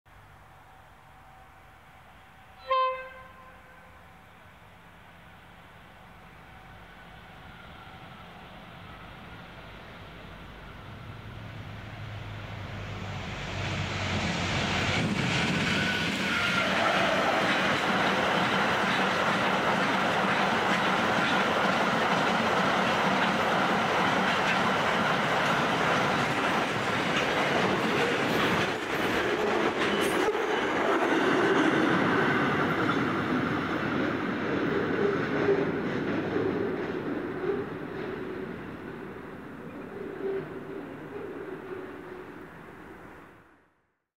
7922V SCT / SBR Dooen Container Train With CSR009 (24/7/2023) - PoathTV Railways
The Wimmera Container Lines service operated by SCT / SBR, running from Dooen (Horsham) to Melbourne, approaches the Buchter Road railway crossing shortly before joining the dual gauge track in Batesford.

Locomotives / Consist:  CSR009, 27 wagons.